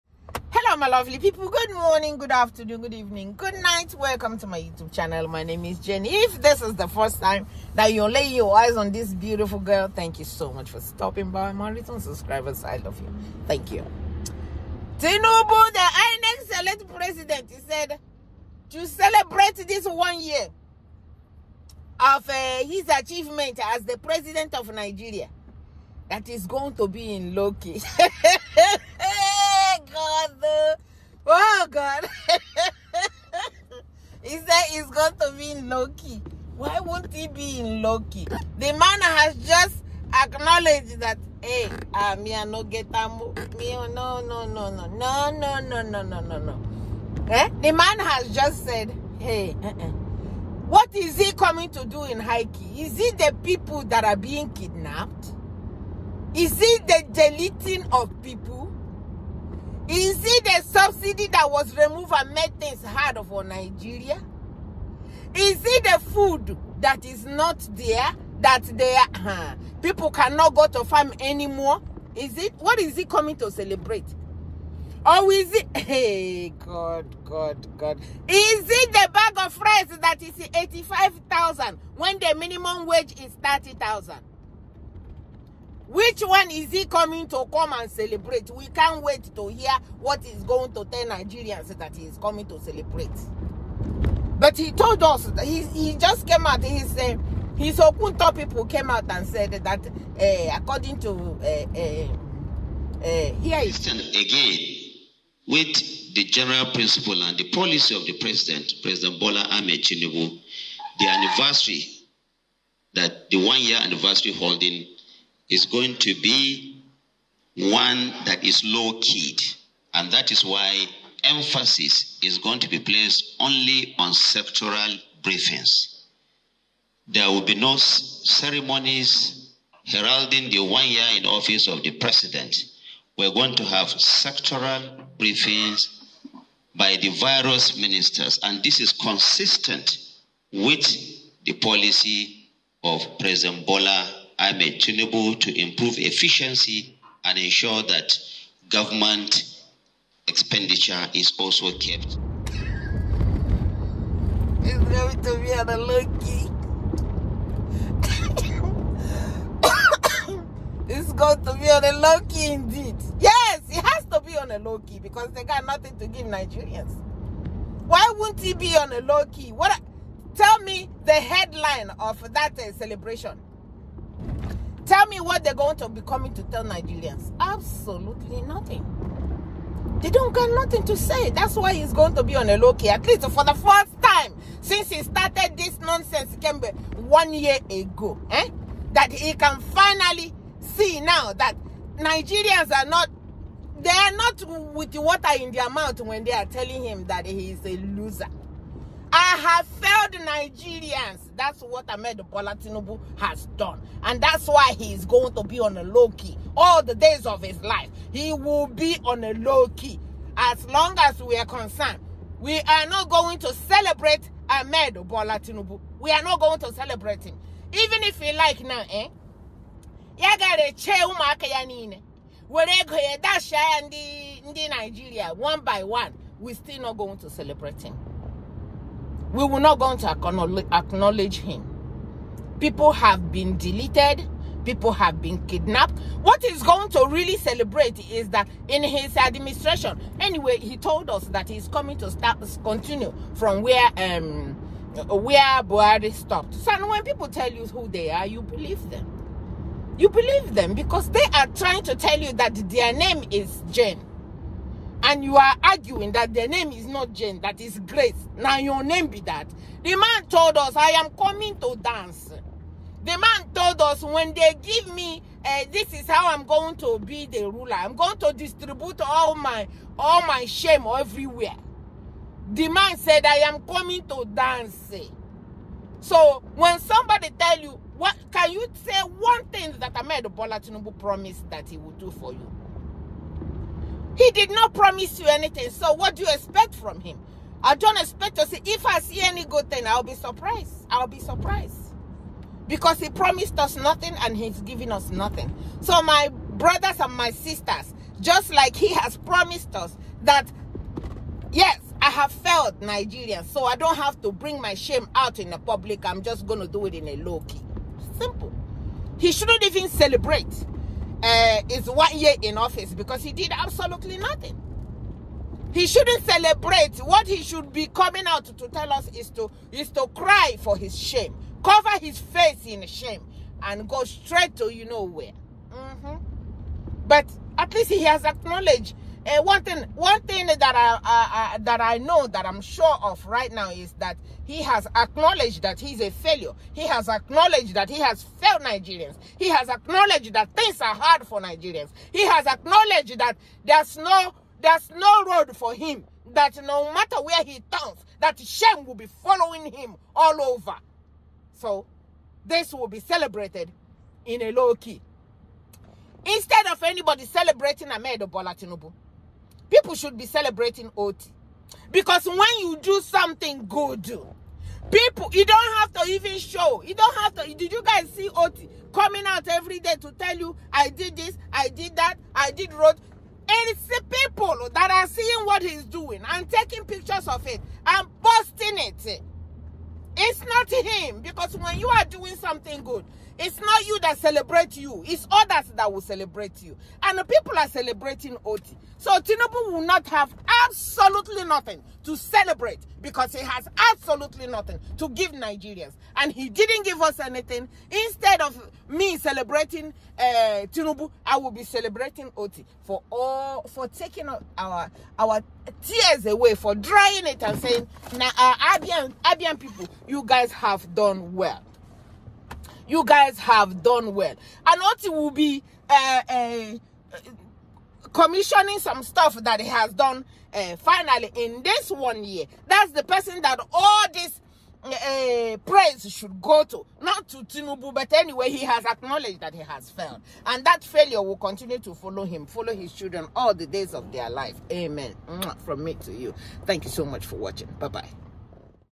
🤪😎 INEC SELECT PRESIDENT WILL CELEBRATE HIS ONE YEAR IN OFFICE IN A LOW KEY 😂
INEC select President of Nigeria will  show Nigerians what he has done in 1 year 😂😎